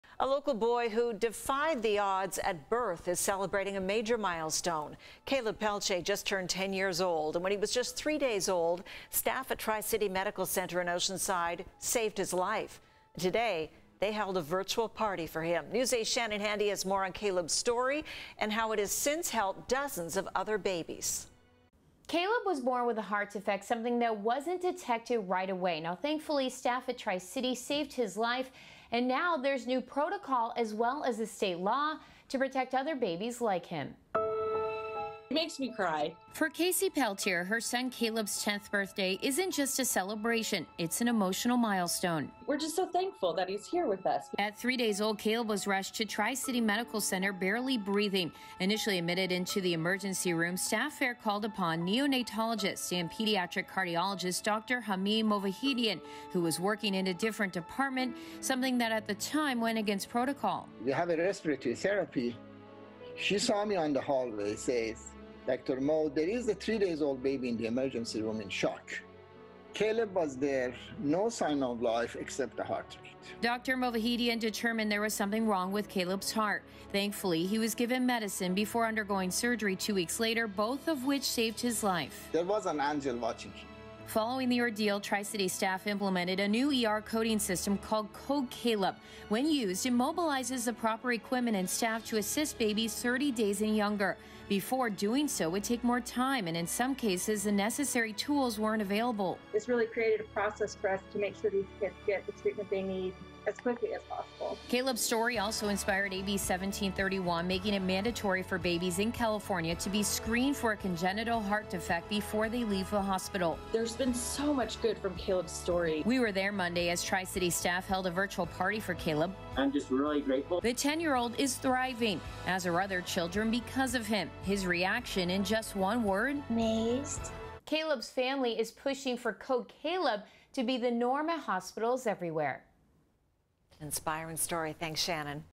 10 years later, miracle baby reunites with Oceanside hospital workers that saved his life
Staff at Tri-City saved his life.